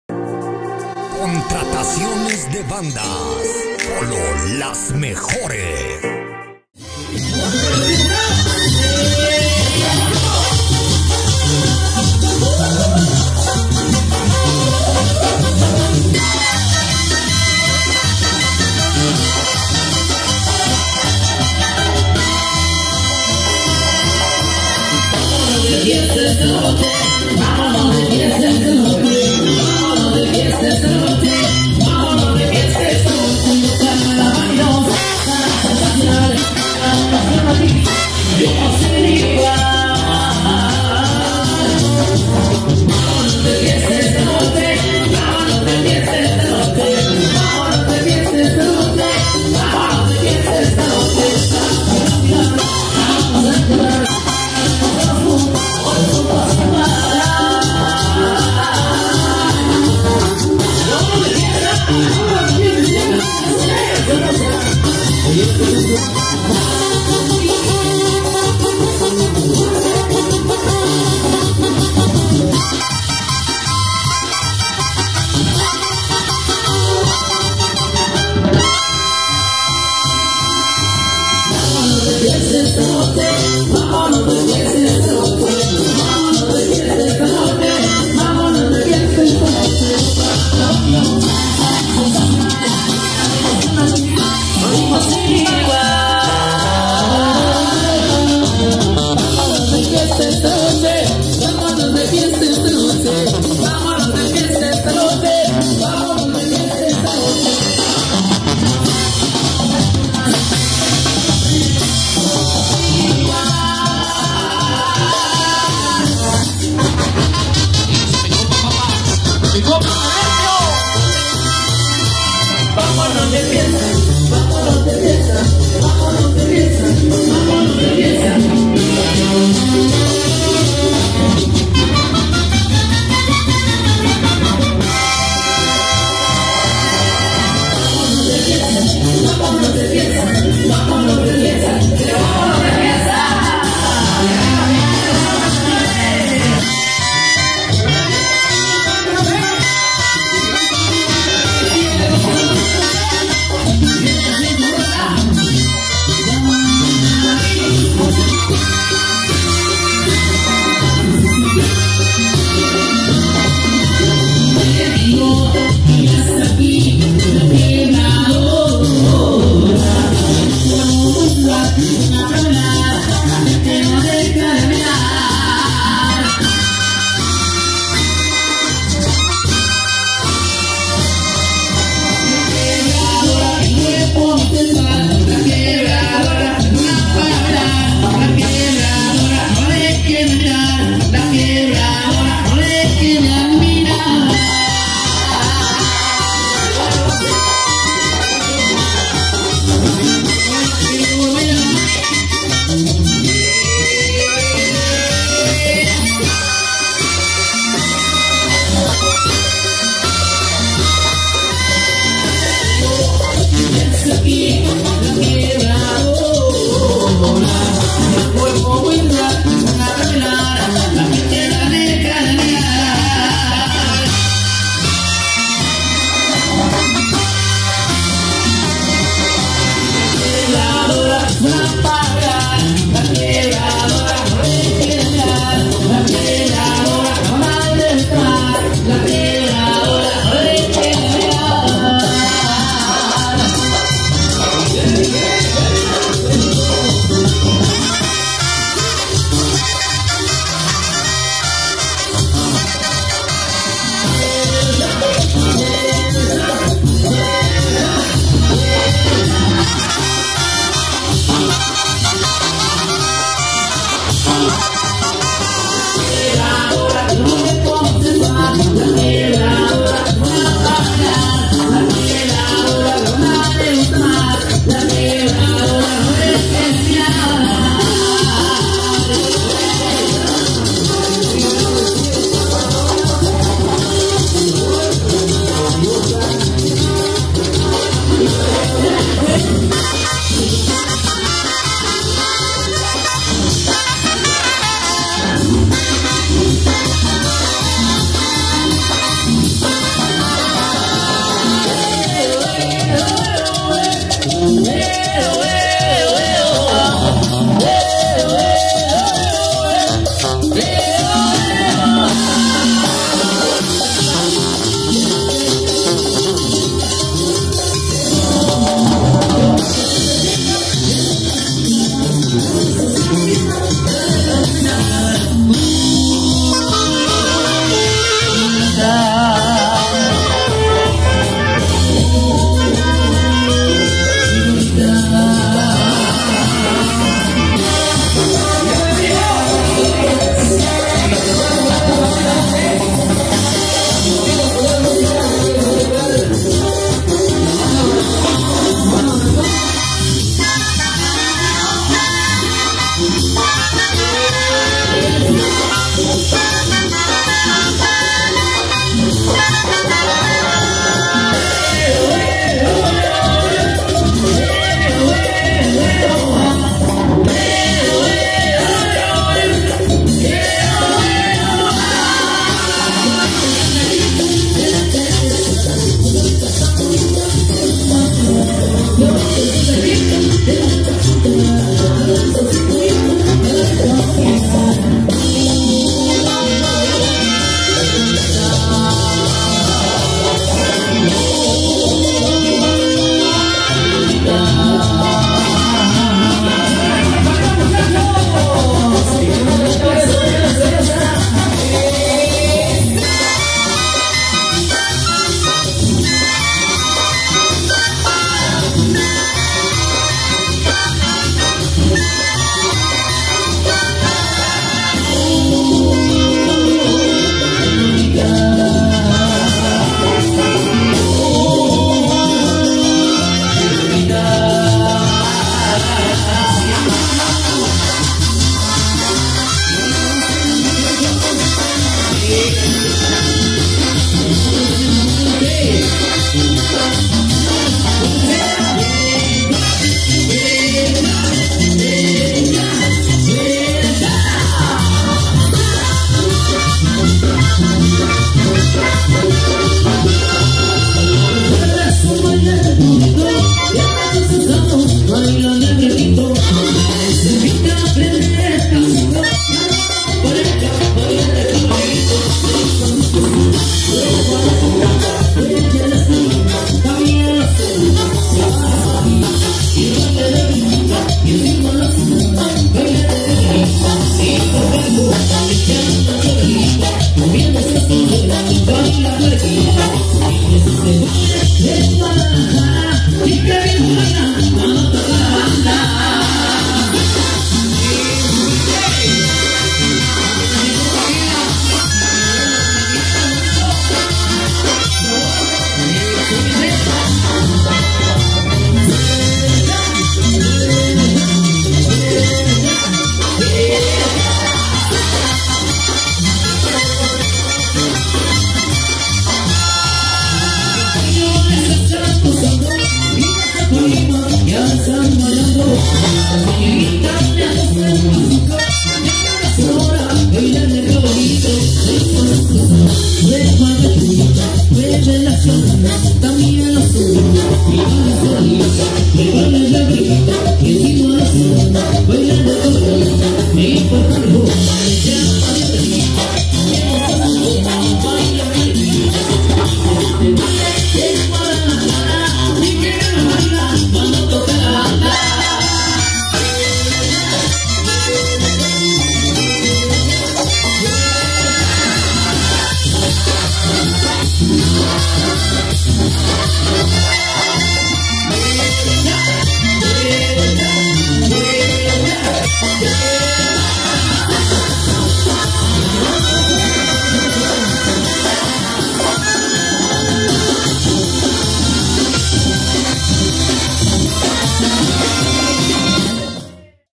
Popurri del Recodo (en vivo) Orgullosamente Banda Vanidosa
desde USA:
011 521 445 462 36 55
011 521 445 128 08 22

EN VIVO! Popurri de "BANDA EL RECODO" la orgullosamente BANDA VANIDOSA de San Agustín del Pulque municipio Cuitzeo, Michoacan.